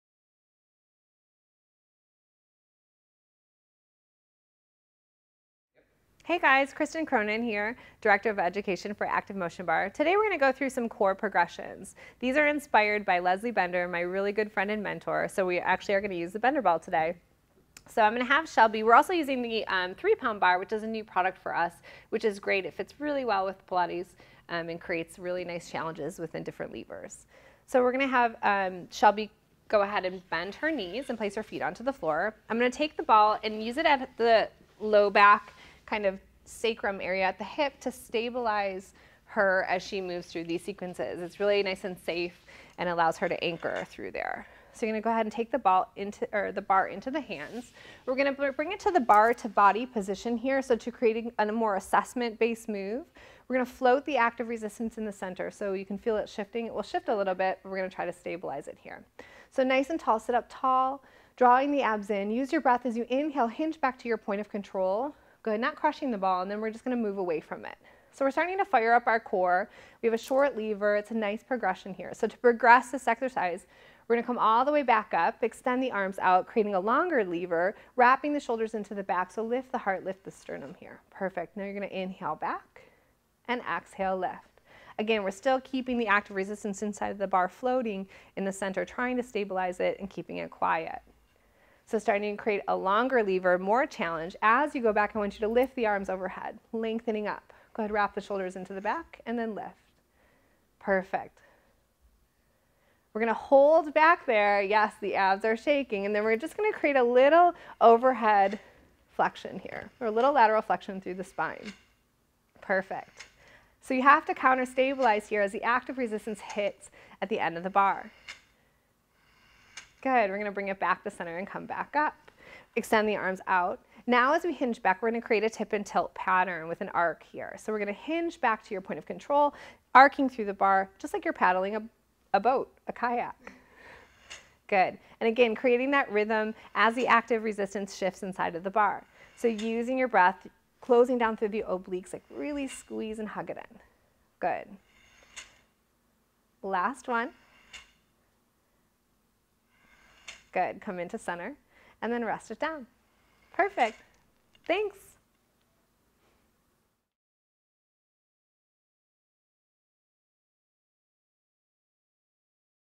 ActivMotion Bar Pilates Earthquake
Try this great Pilates Mat exercise with your ActivMotion Bar and Bender Ball and elegantly rock your body to the core!